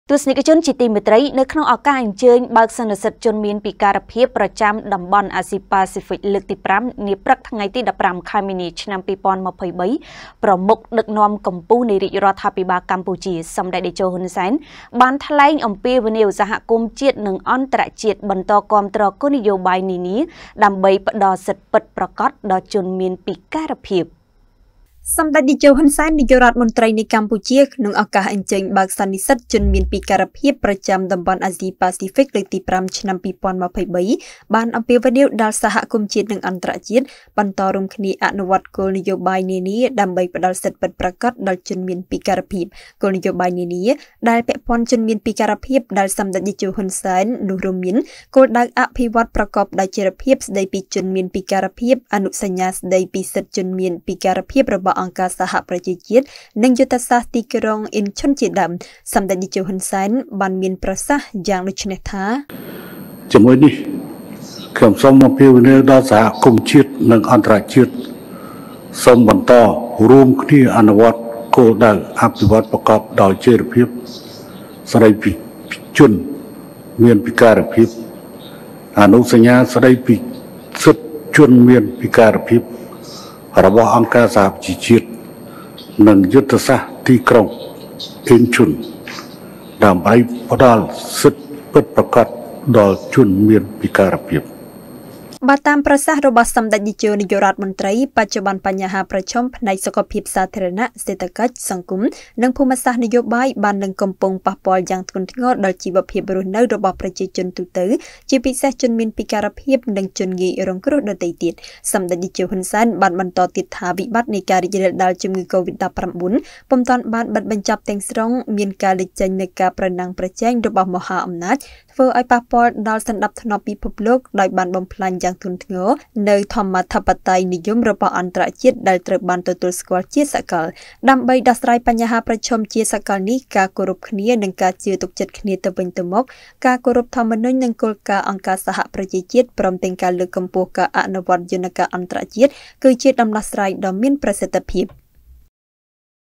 សម្ដេចតេជោ ហ៊ុន សែន អំពាវនាវសហគមន៍ជាតិ និងអន្ដរជាតិ បន្ដគាំទ្រគោលនយោបាយនានា
សម្ដេចតេជោ ហ៊ុន សែន អំពាវនាវសហគមន៍ជាតិ និងអន្ដរជាតិ បន្ដគាំទ្រគោលនយោបាយនានា ដើម្បីផ្ដល់សិទ្ធិពិតប្រាកដដល់ជនមានពិការភាព
* រាយការណ៍ដោយកញ្ញា  យឹម ចាន់ស្រីវីន​ និងកញ្ញា សំ ស្រីនីត